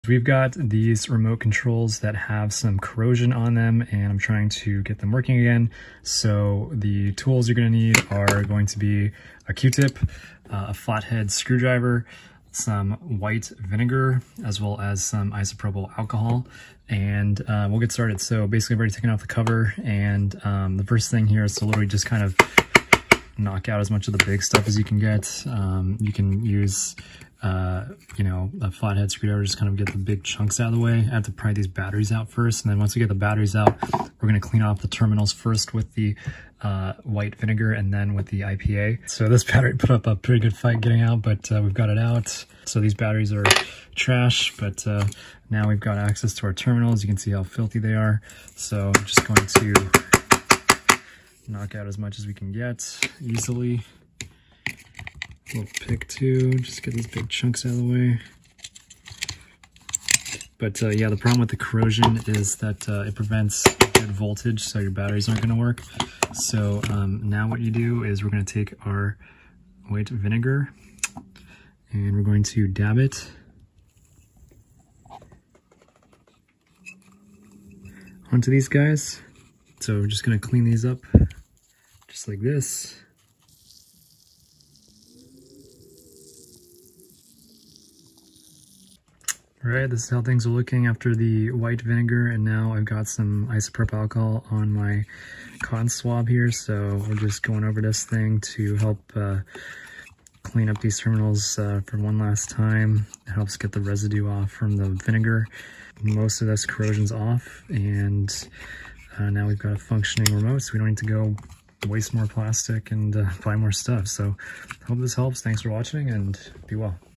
How to clean corroded battery terminals in electronic devices at home - fast and easy!
I show how to clean corroded battery terminals in electronic devices like TV remotes at home using white vinegar and isopropyl alcohol. It's fast and easy, and you can do it right now at home!